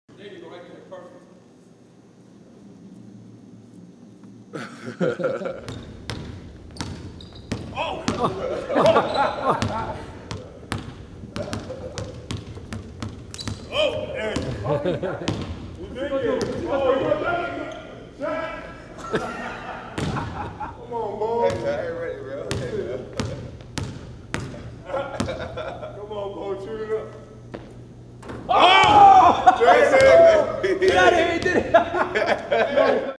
The Champ is Here
Al Horford goes 1-on-1 with UFC Light Heavyweight Champion Jon "Bones" Jones.